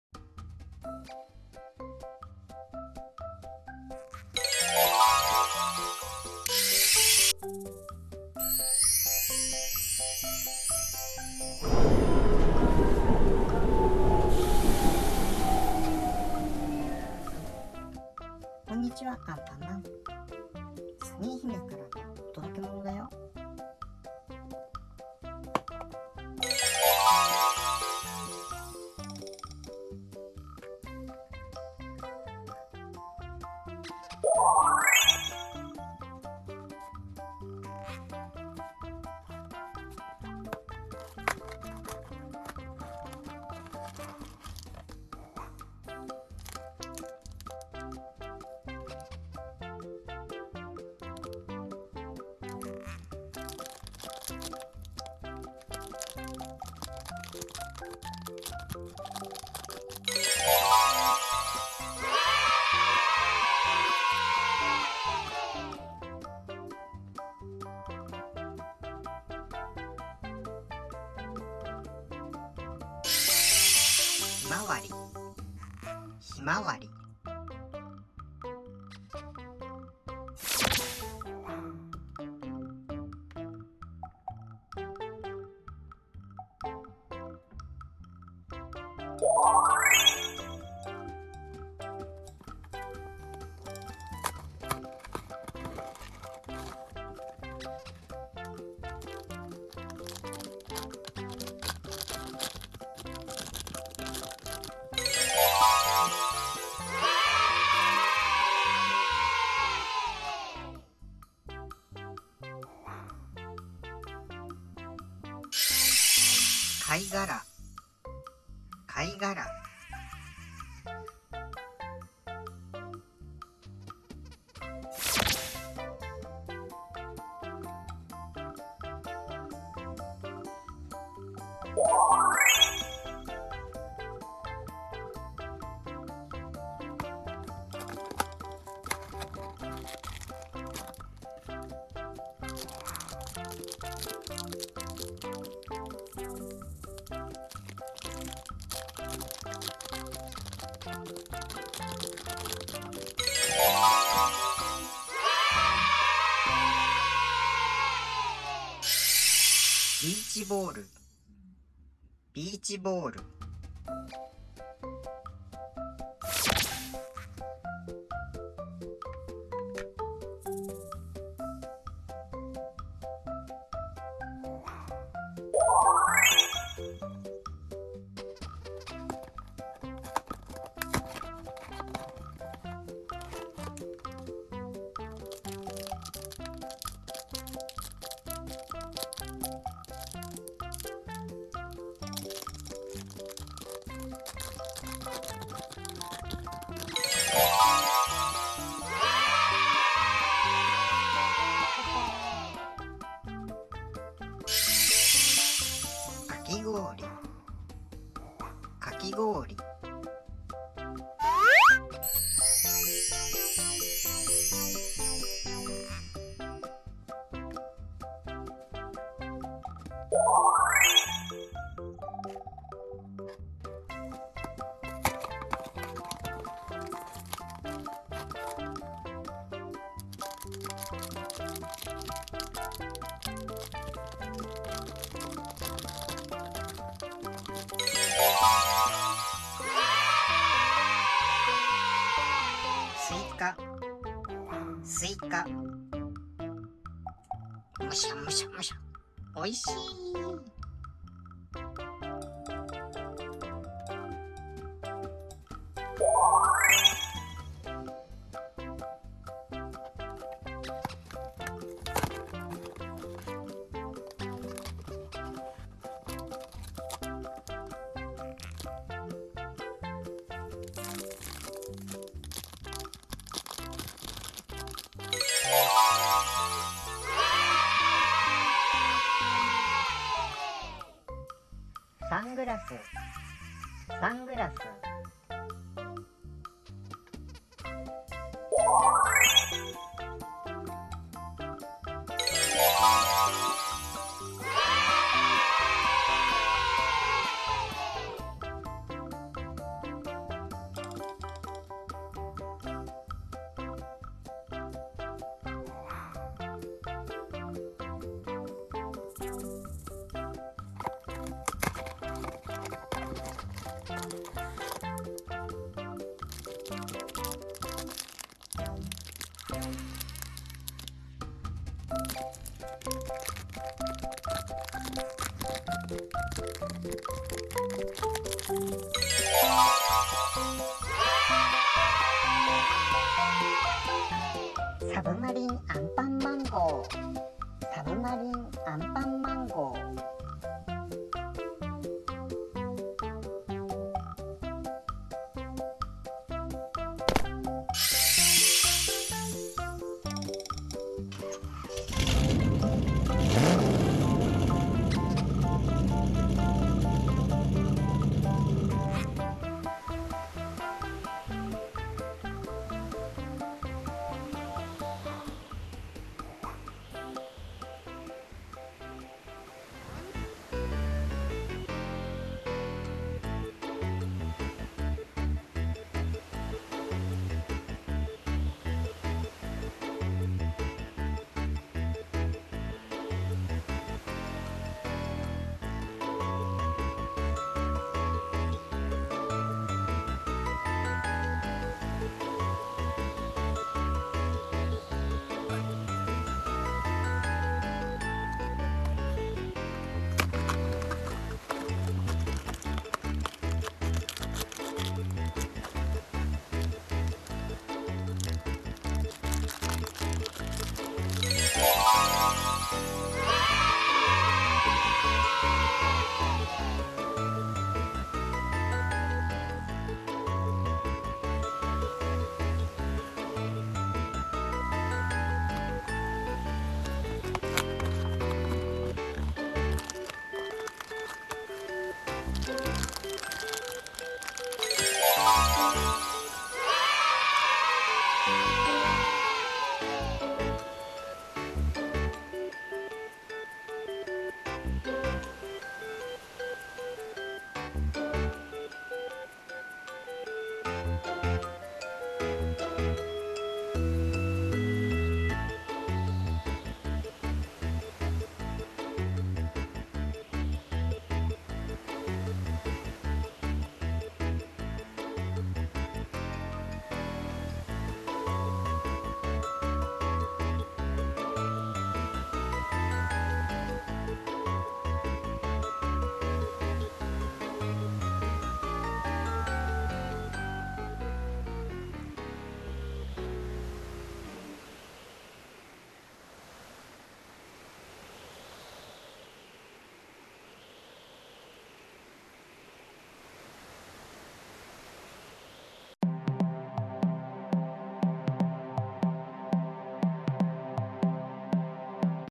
あつまれアンパンマン74『アンパンマンと海の仲間たち』知育動画 食玩
大人気指人形あつまれアンパンマン74です
みんなでうみであそんだよ。
夏にまつわることばおぼえ。
アンパンマン/ばいきんまん/コキンちゃん/あかちゃんまん/ポッポちゃん/クリームパンダちゃん/サニーひめ/ベソ/サブマリンアンパンマンごう
以上今回のシリーズ
ドキンちゃん/SLマン/ちびマリン

音楽などは概要欄にあります